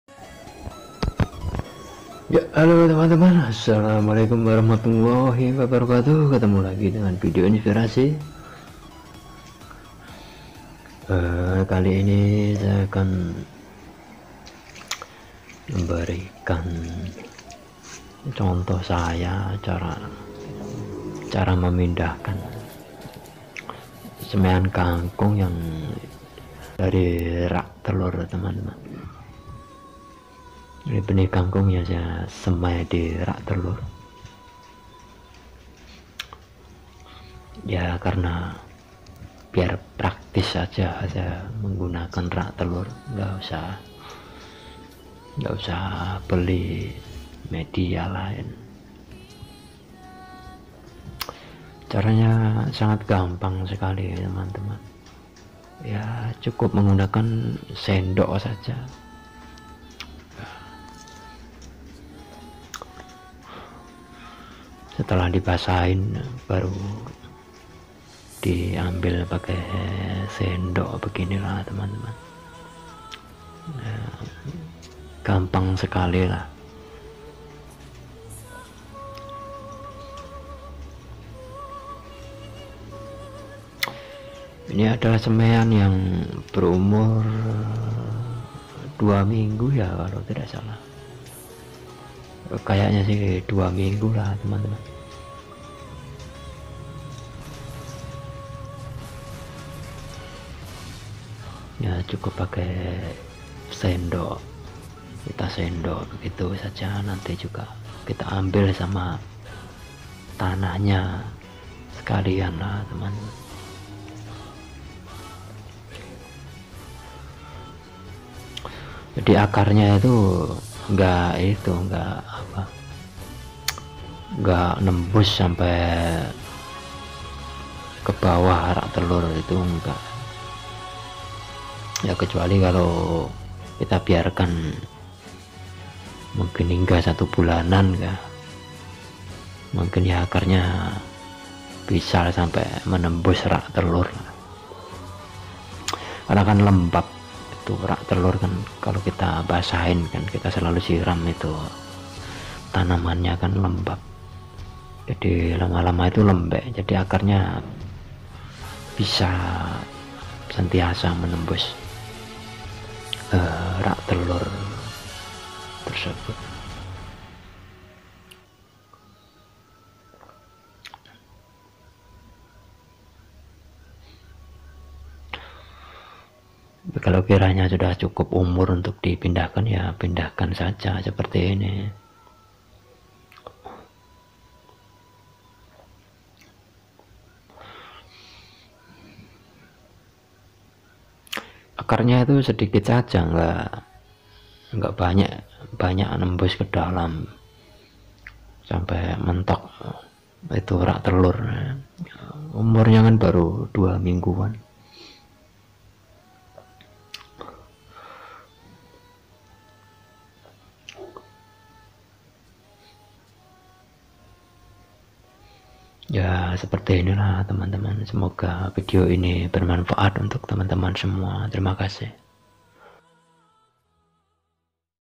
Memindahkan semaian kangkung dari RAK TELOR
Cukup menggunakan sendok utk mengambil semaian dari RAK TELOR,yg sblmnya basahi terlebih dahulu semaian nya, utk memudahkan proses pengambilan semaian kangkung agar tdk rusak akarnya, dan tanahnya bisa terangkat